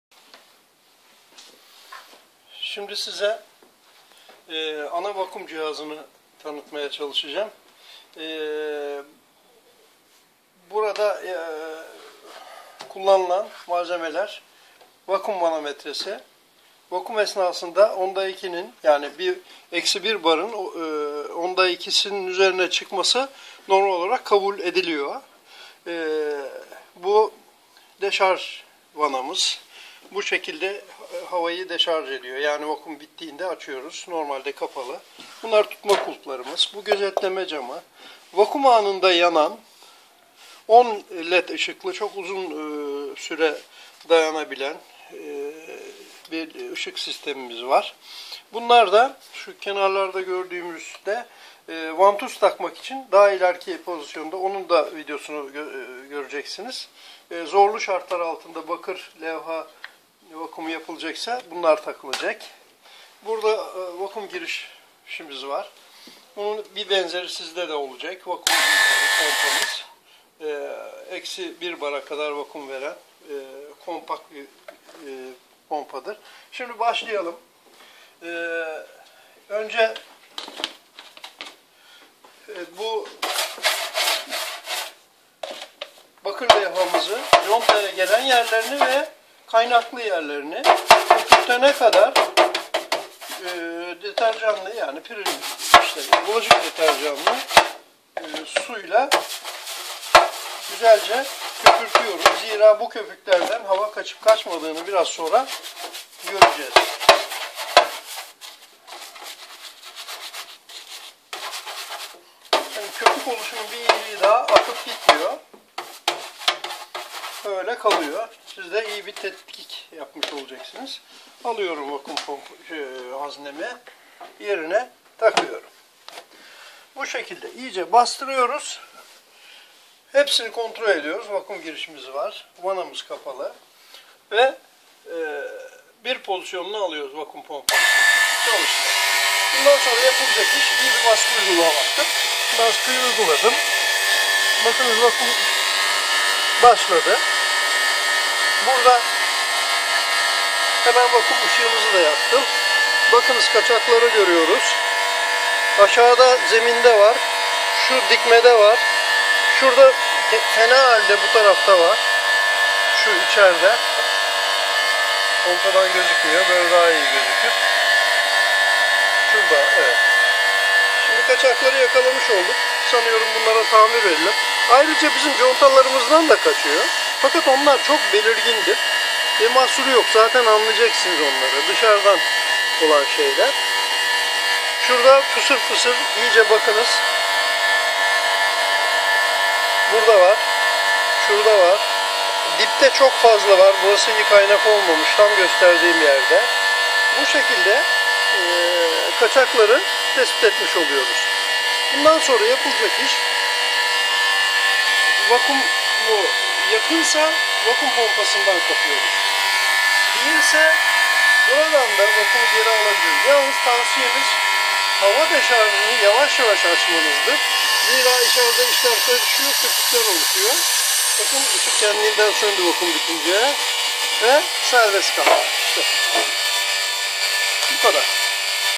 Ilısu barajı vakum test cihazı
Hasankeyf Ilısu barajı su tutucu  levhalarının (bakır) kaynak olmuş ek yerlerinin  kalite kontrolu için geliştirilmi bir vakum haznesidir.Şekli özel olarak bükülmüş bakır su tutucu levhalarına göre dizayn edildi test edildi şu baraj inşaatında başarıyla kullanılmaktadır.